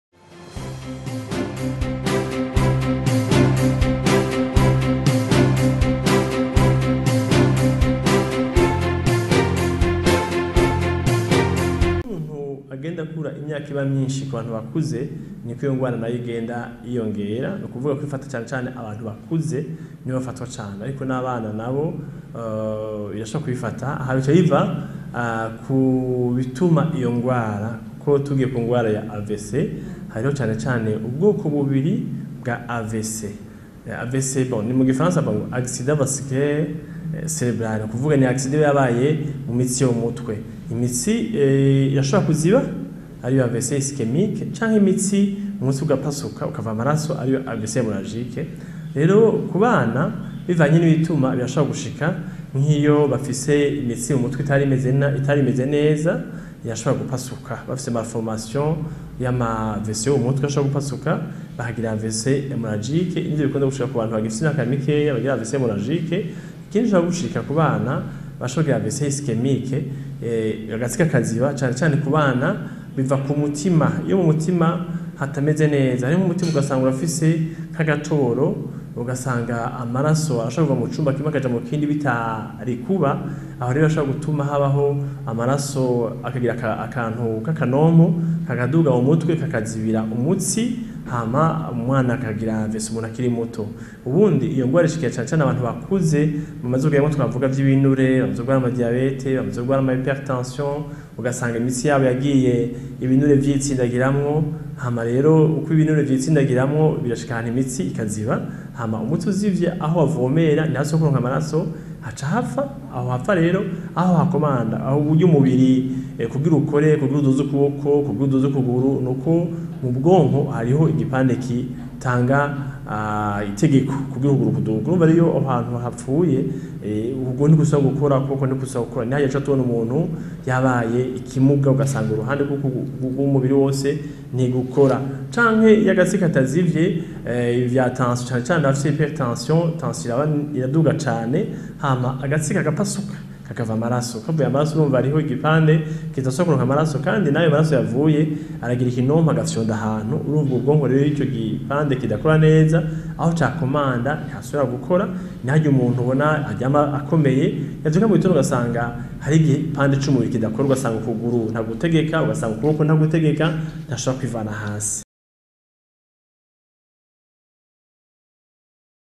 AVC ni indwara ivurwa igakira mu gihe ivuwe kare (Dr J. Nzisabira)
Abantu bariko barakura bategerezwa kwirinda ibibatera indwara y’umutima bita AVC kuko uko umuntu akura ari ko vyoroha ko ayandura, n’aho n’abana bishika bakayirwara. Bisigurwa na muganga w’umuhinga avura indwara z’umutima Joris Nzisabira. Iyo ndwara ikaba iterwa n’ibintu vyinshi bitandukanye. Ariko kandi agahumuriza ko iyo ndwara iyo ivuwe kare ikira.